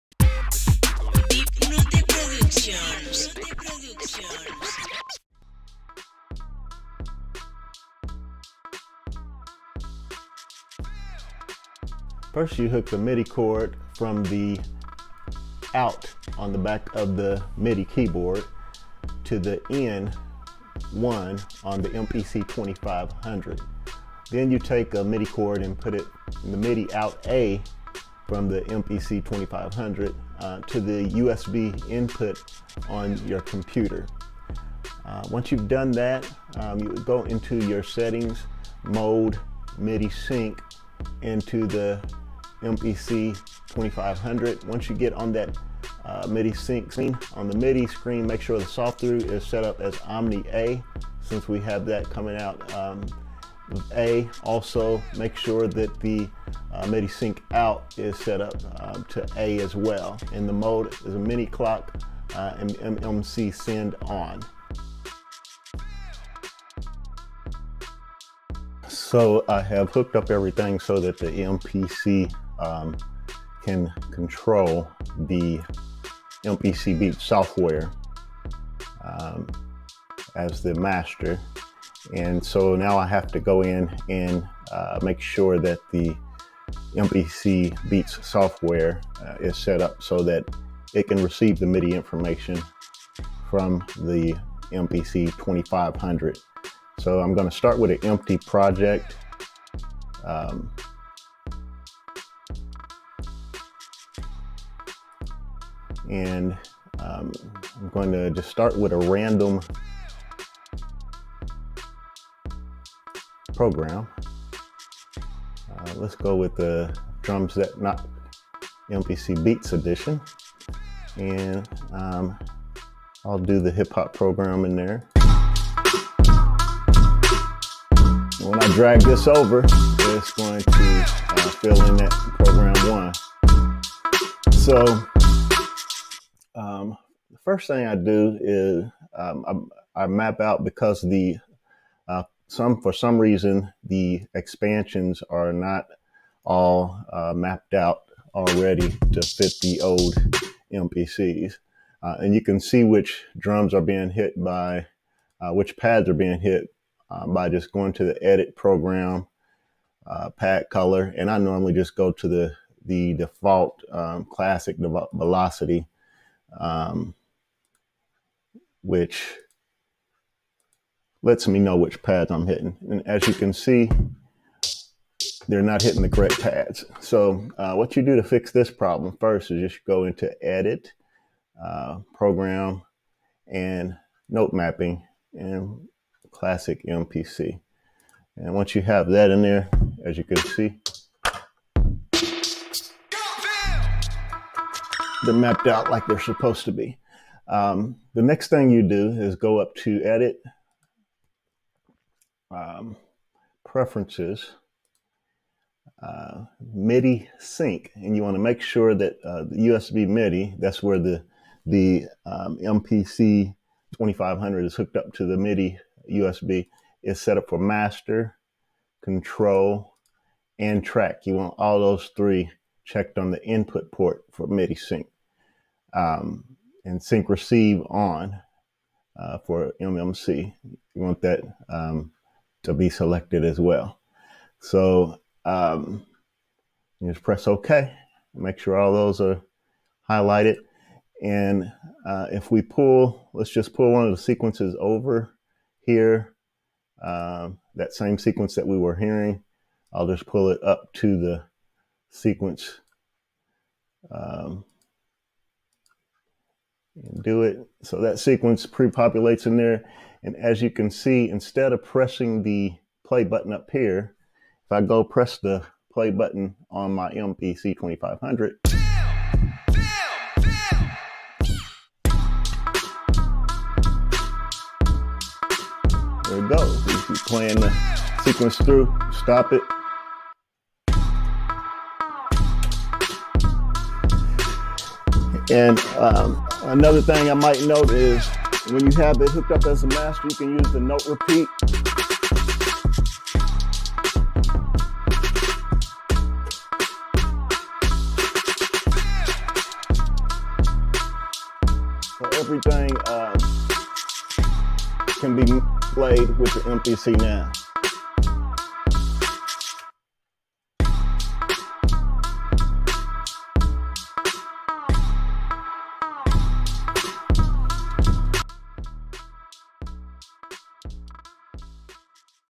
MPC 2500 x MPC Beats - Using Akai MPC 2500 as Master w/Midi Keyboard Controller - Tutorial
MPC 2500 x MPC Beats - Using MPC 2500 as Master w/Midi Keyboard Controller - Tutorial

This video demonstrates how to use an Akai MPC 2500 to control Akai MPC Beats software with a midi keyboard running through it. This is an MPC 2500 tutorial and MPC Beats set up tutorial. How to use an old school MPC with MPC software.

Timestamps

0:00 - Connecting the MIDI Cables
0:40 - Internal Midi Settings on MPC2500
2:25 - Mapping MPC2500 Pads to MPC Beats Software
3:20 - MIDI Settings in MPC Beats Software 🎶All things Deep Note